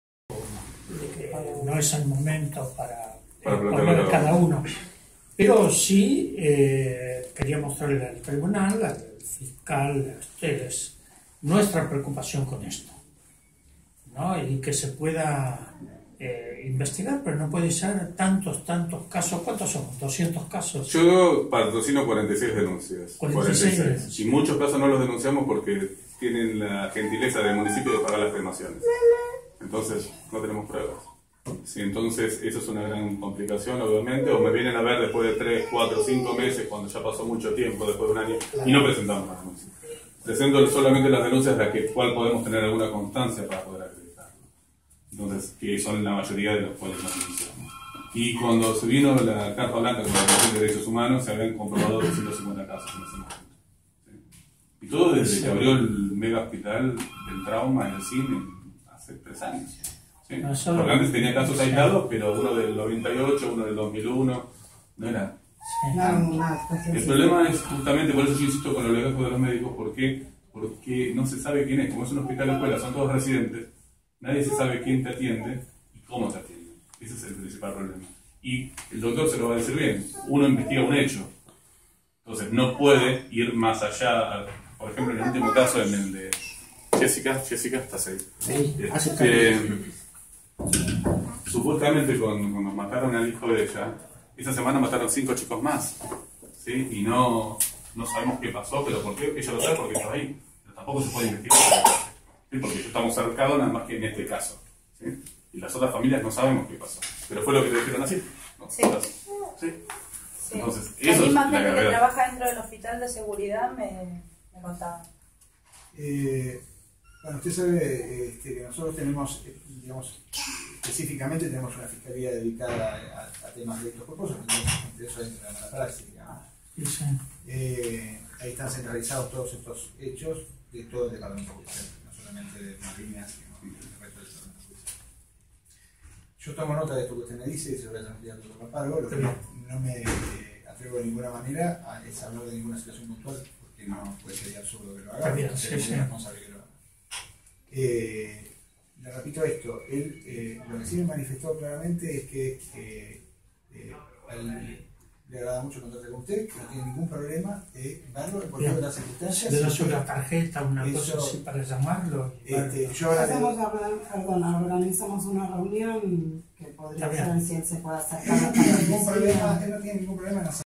Adolfo Perez Esquivel visito a los fiscales que atienden los casos de mala praxis
APCS. El Premio Nobel de la Paz, Adolfo Perez Esquivel, visito a los fiscales que atienden los casos de mala praxis de Malvinas Argentinas junto a la Comisión de Familiares de Mala Praxis y el abogado Jorge Cancio.